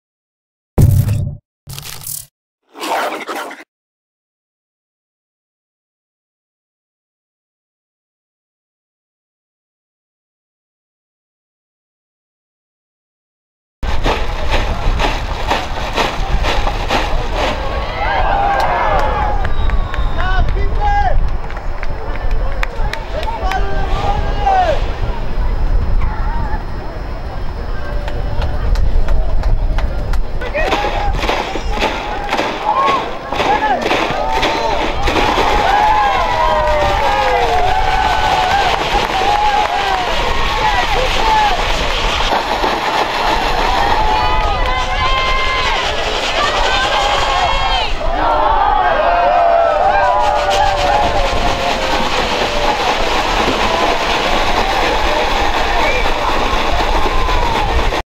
Dallas City FC vs Forth Worth Vaqueros - Mozzo save
Dallas City FC vs Forth Worth Vaqueros - Mozzo saves some tough shots